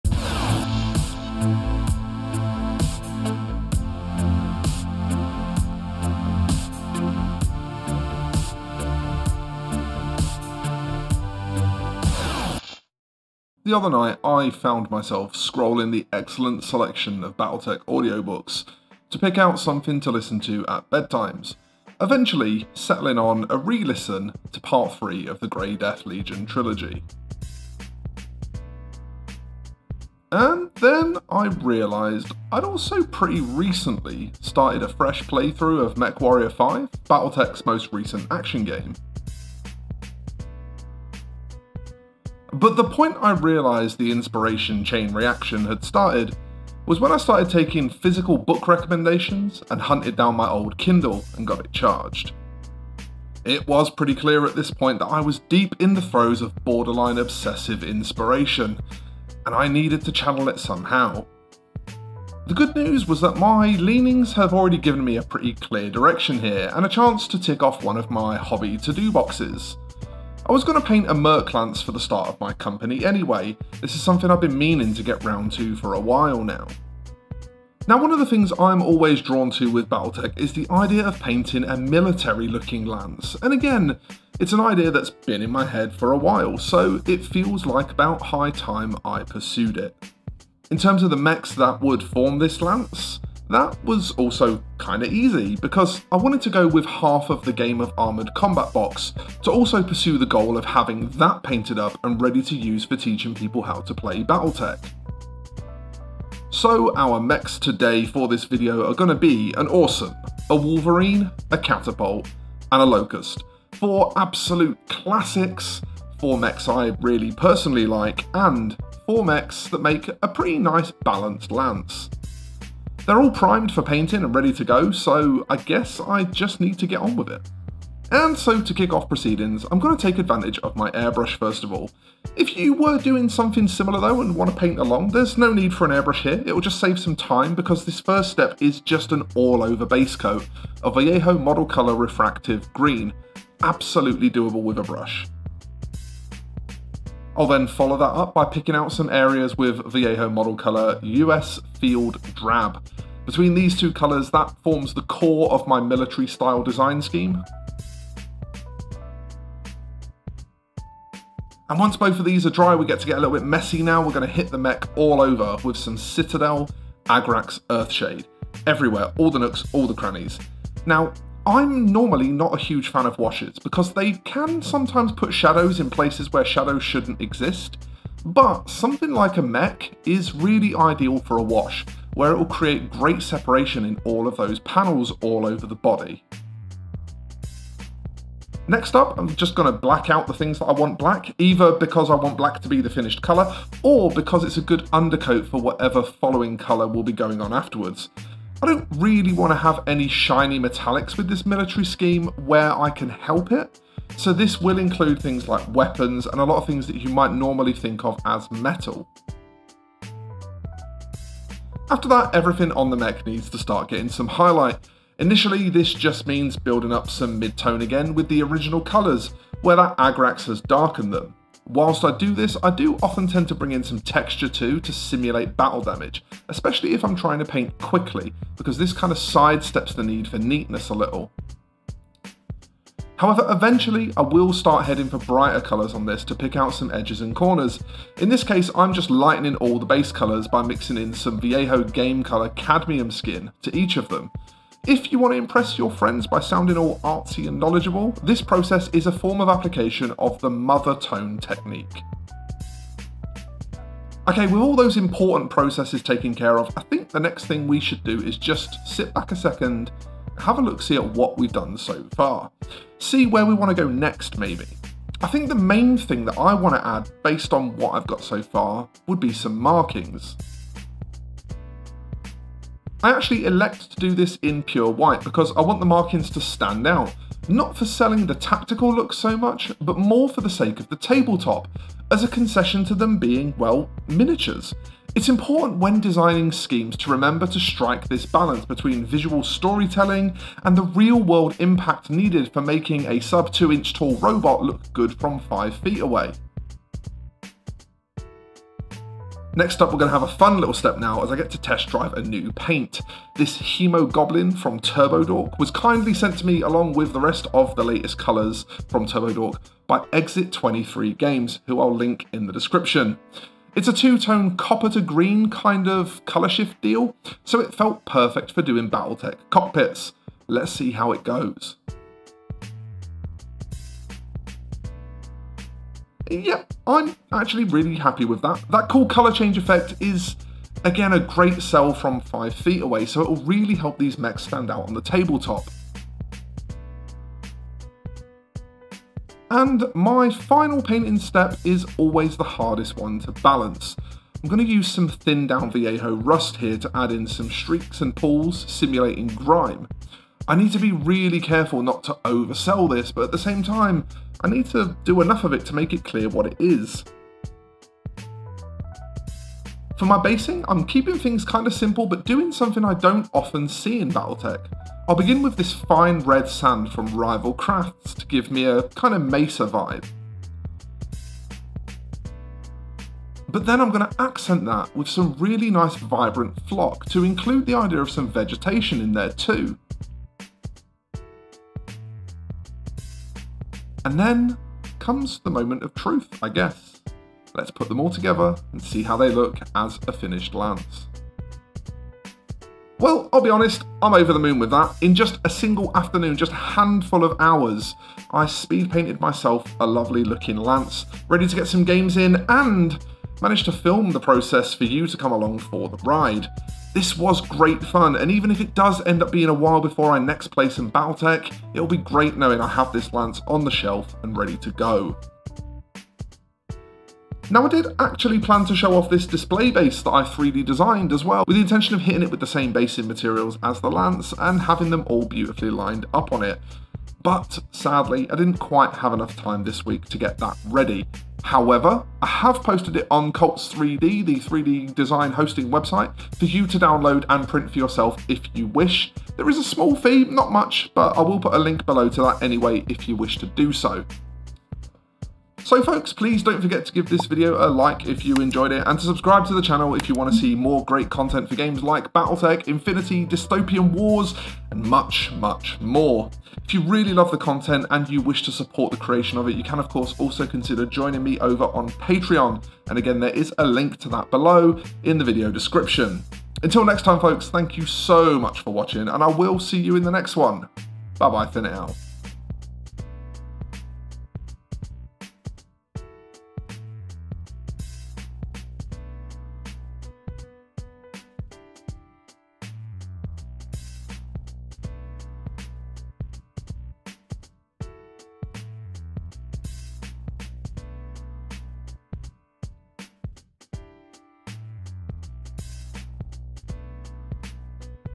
Battletech: my ULTIMATE Inspiration! (Painting a Mercenary Lance)
Today we go on a journey, that all started with uncontrollable inspiration... Come along with me as I go through how my first Merc lance came to be.